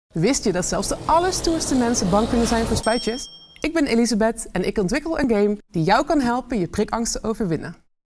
Nominatie Klokhuis Wetenschapsprijs 2021: Overwin je angst voor spuitjes met een spelletje! (8sec)
Dr. Elisabeth Huis in ’t Veld is genomineerd voor de Klokhuis Wetenschapsprijs 2021. Haar onderzoek gaat over prikangst.

Wil je daar meer over weten?